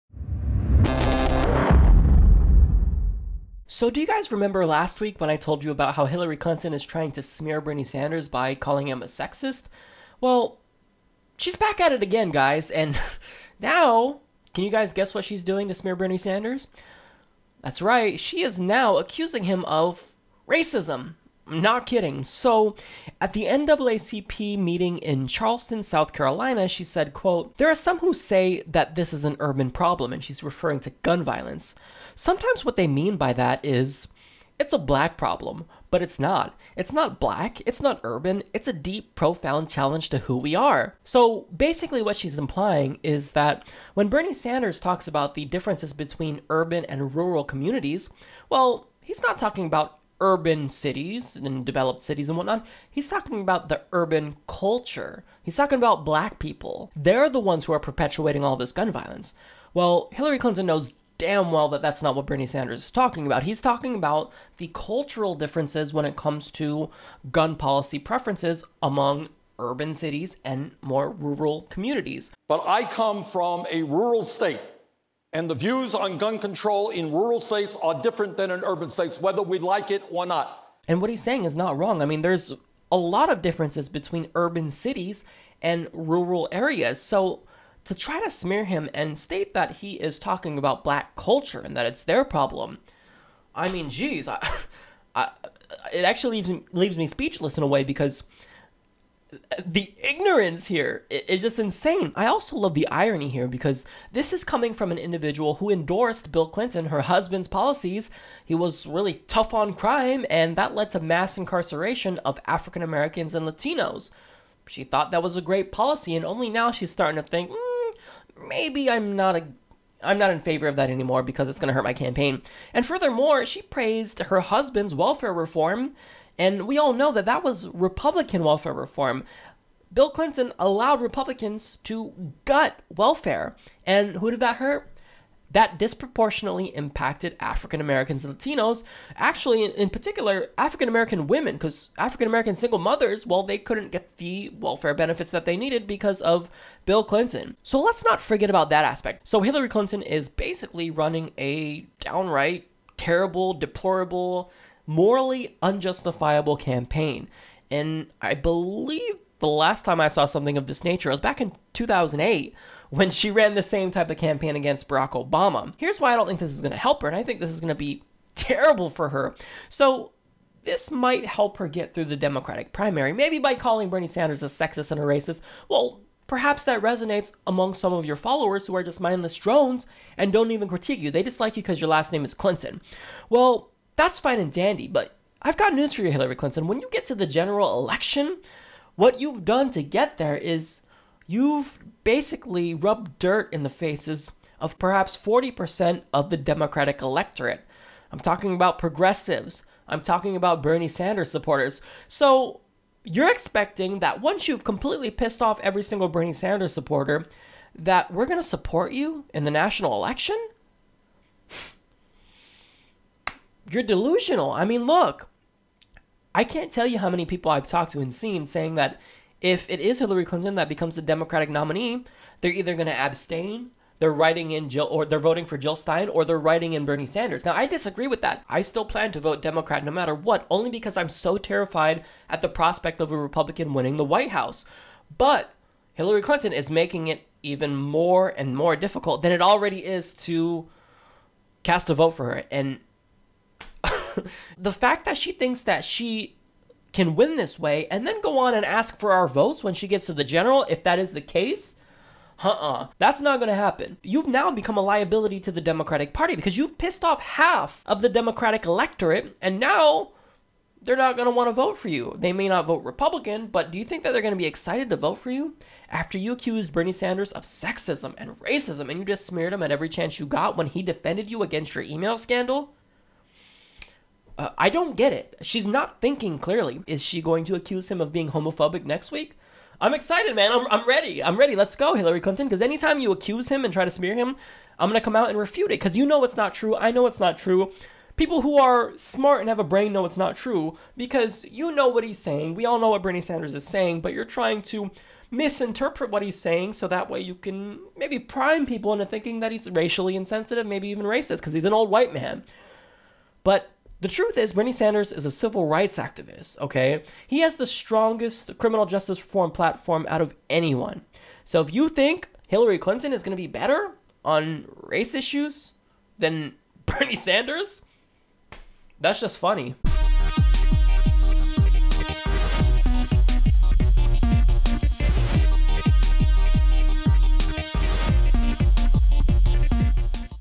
Hillary Clinton Slanders Bernie Sanders Again & Implies He's Racist
Last week Hillary Clinton smeared Bernie Sanders and accused him of being a sexist. This week, she is accusing him of racism because he stated that there are differences between urban cities and rural areas when it comes to gun policy preferences. Hillary attempted to ascribe that comment to racial insensitivity. We refute this ridiculous accusation in this video.